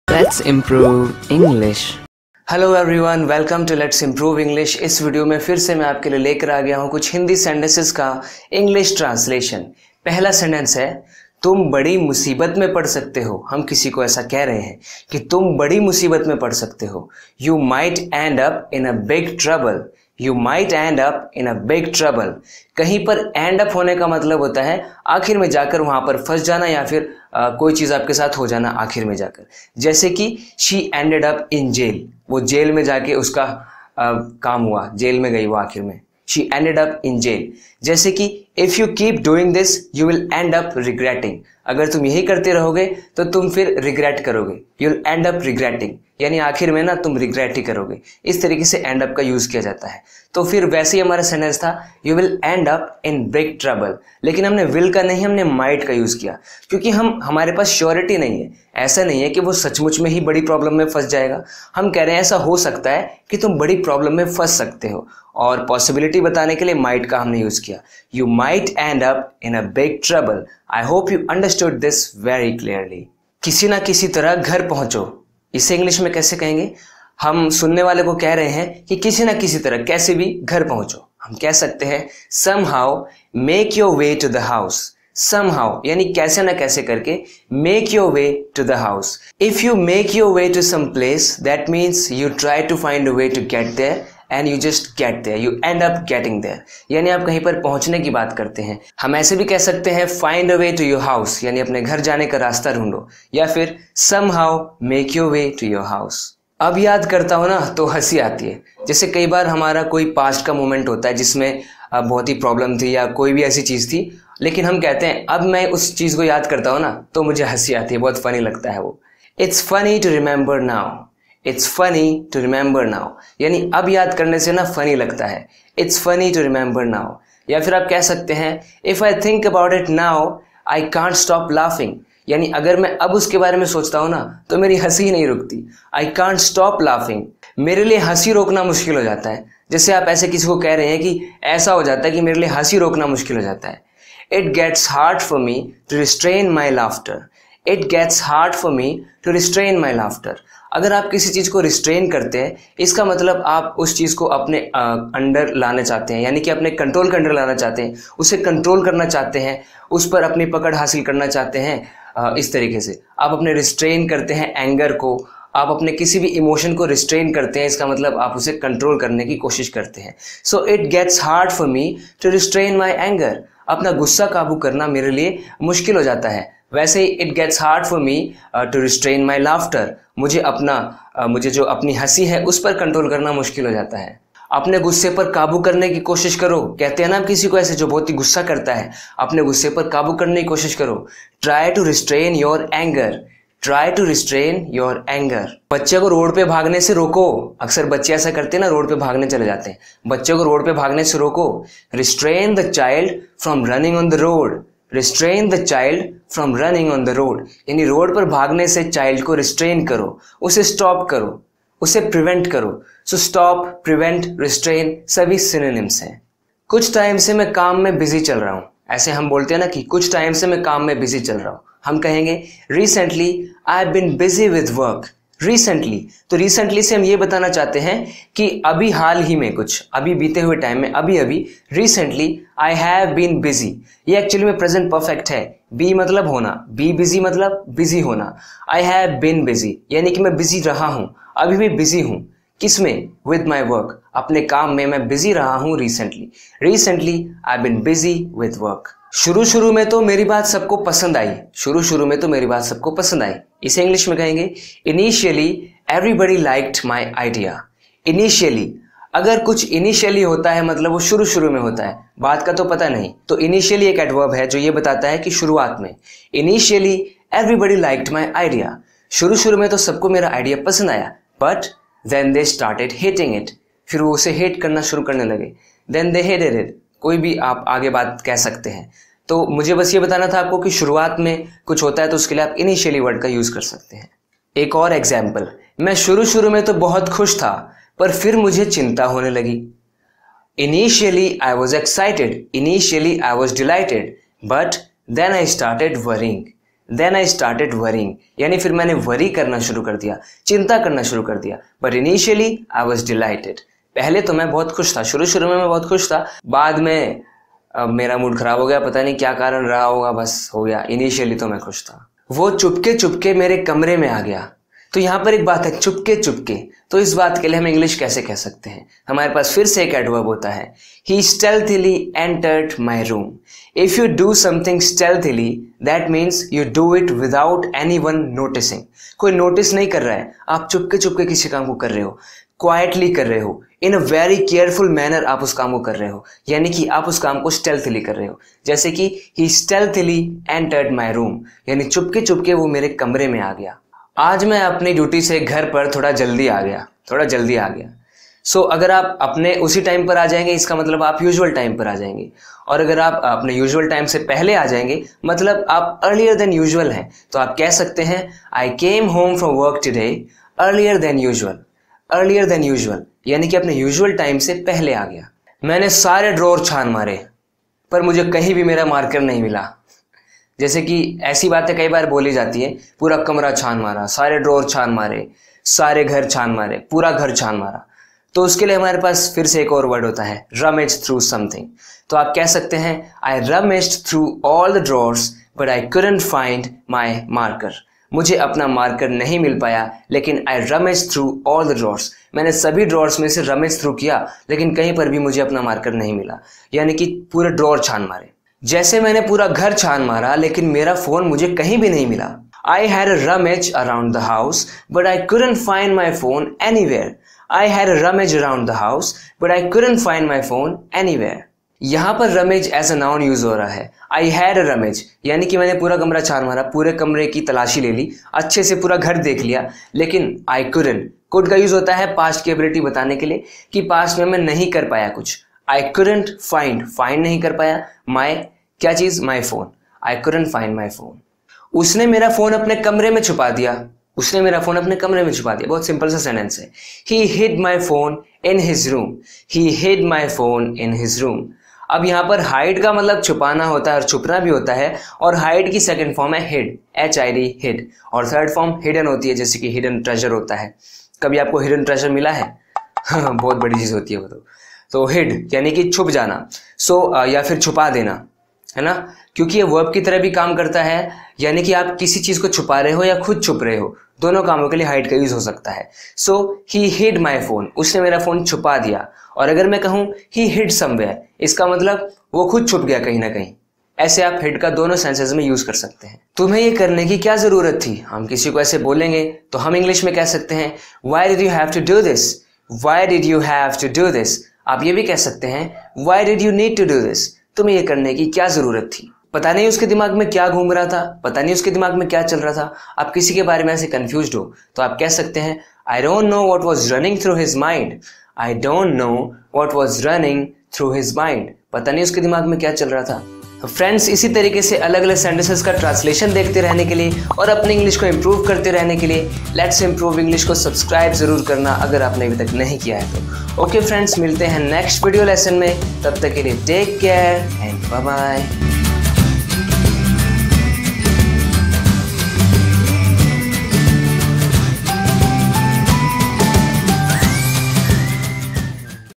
Daily Use English Sentences | Meaning of Stealthily | Hindi to English Translation Exercise
In this video lesson, we are going to do a Hindi to English translation exercise. You are going to learn a lot from this video.

Keep on learning and improving your English!😊